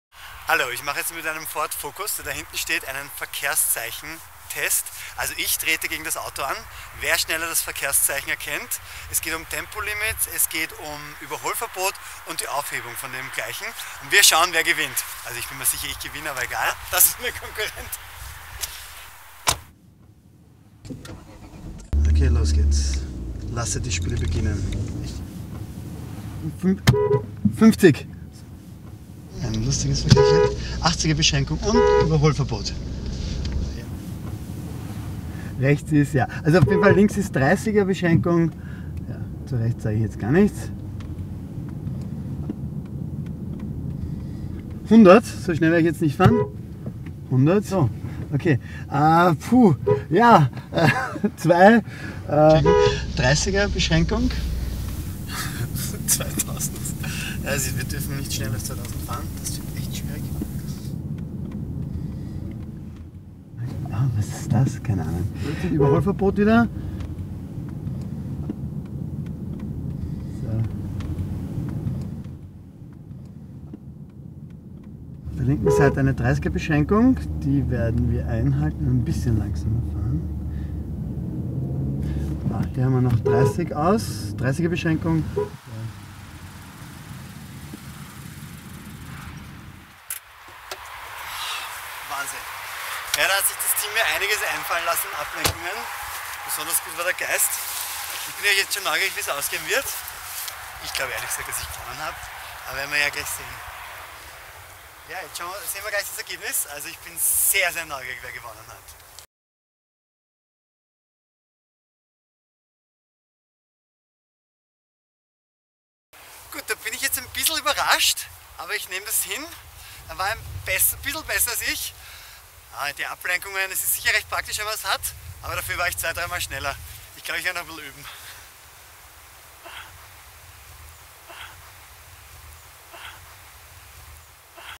Ford Real World Challenge
Andreas Ferner als Testfahrer für Ford Real World Challenge